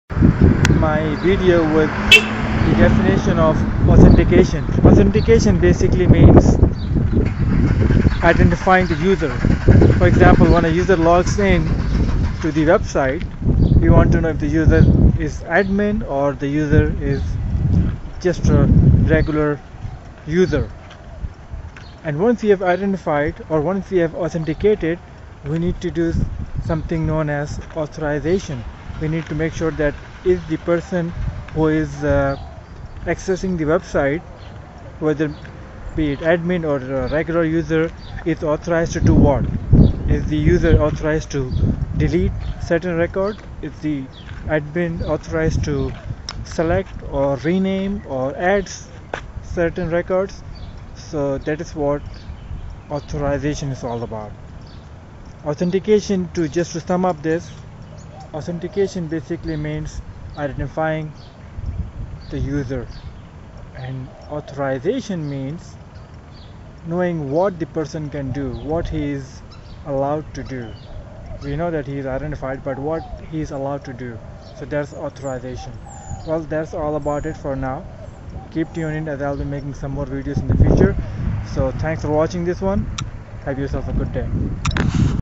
What is Authentication and Authorization
This video demonstrates what Authentication is and what Authorization is. This video gives a general overview these two terms.